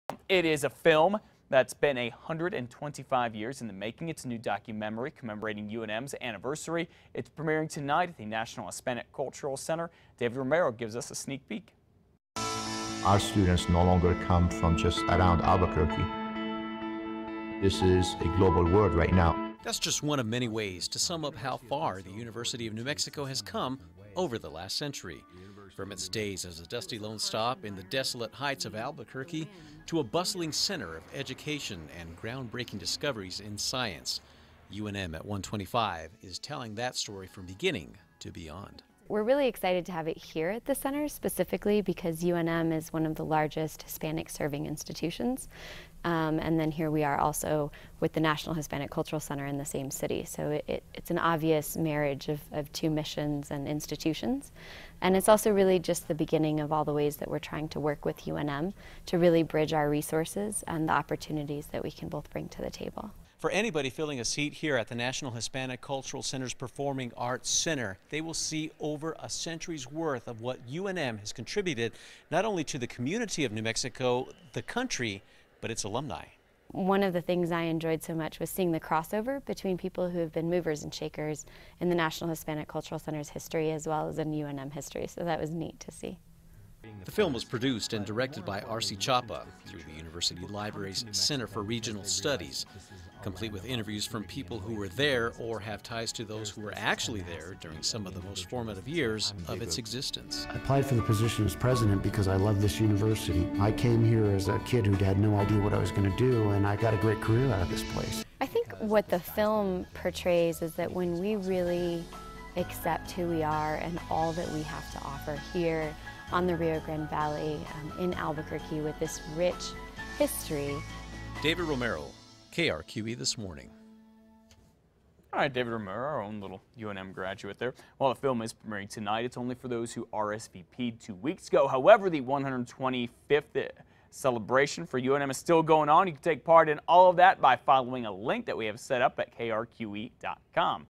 Film documentary premieres celebrating UNM
A new documentary film commemorating the 125th anniversary of the University of New Mexico will premiere on Tuesday night at the National Hispanic Cultural Center.